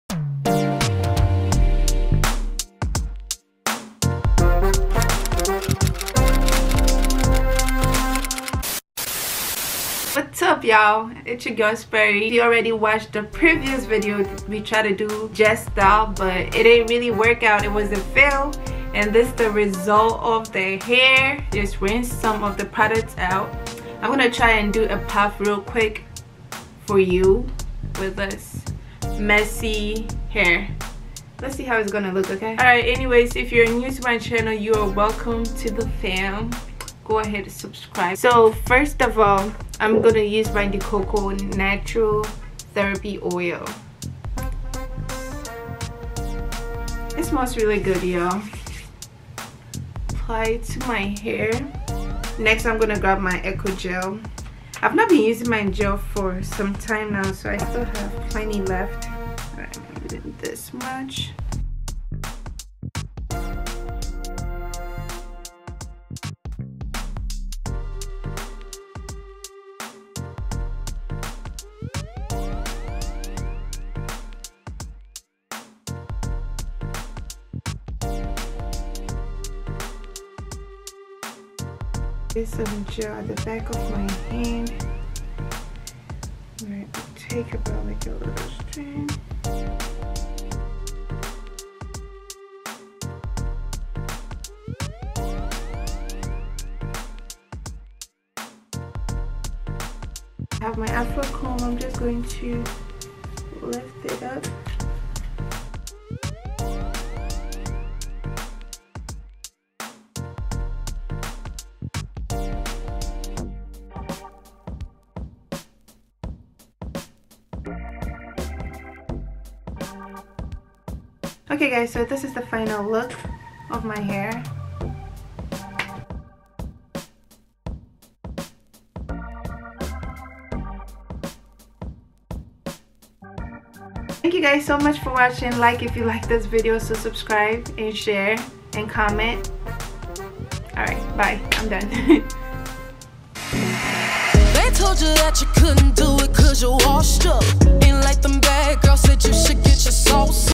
HOW TO MAKE A PUFF ON SHORT 4C NATURAL HAIR
PUFF  TUTORIAL ON MY SHORT 4C NATURAL HAIR. THIS IS EASIEST NATURAL HAIRSTYLE ALMOST EVERYONE WITH NATURAL HAS DONE BEFORE.

Influenster: Sperryvee